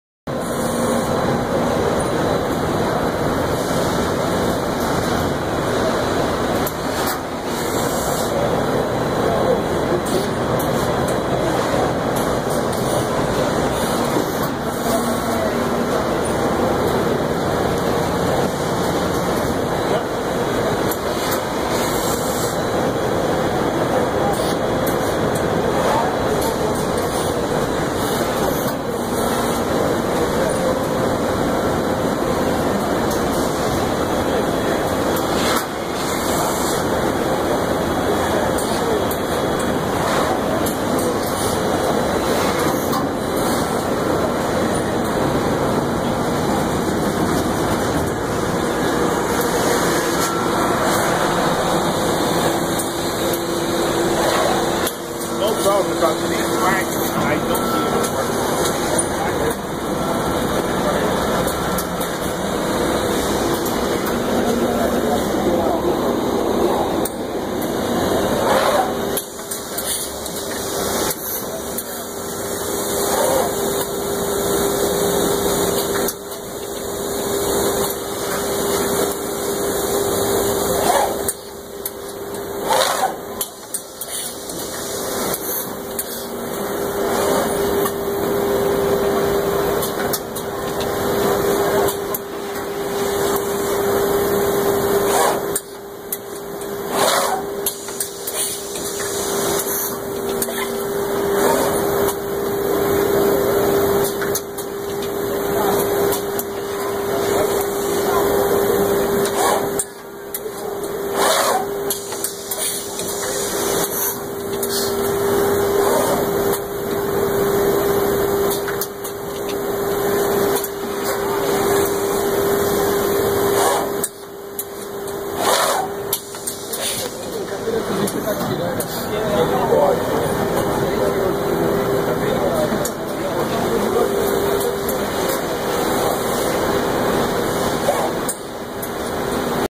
FURNAX - In Mold Label FX
Sistema completo para produção de In Mold Label composto por Injetora Asian Plastic SuperMaster 350TS e Robô Série W8.